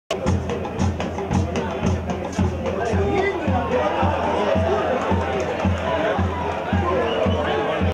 Video Gol: Manuel Moreno 10' (Malacateco) Apertura 2017 Jornada 20
Cortesía: Tigo Sports Guatemala